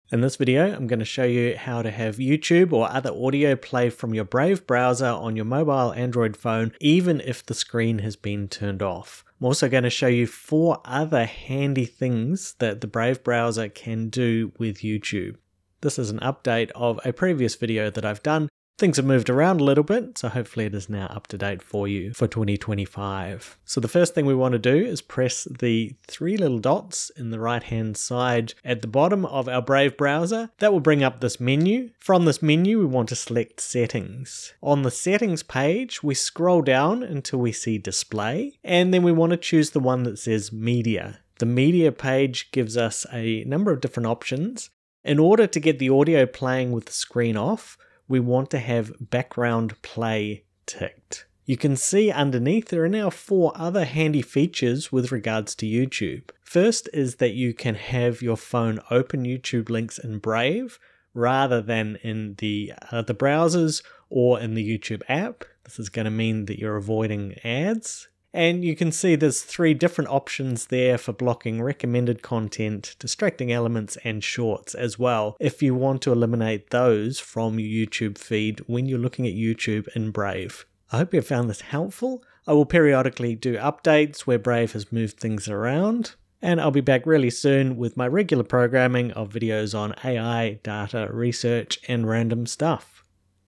Play YouTube in Brave with Screen Off (2025 update)
In this video I show you how to have the audio track from a video (eg. YouTube) keep playing on the Brave browser on your Android phone, even when the screen is off and the phone is locked.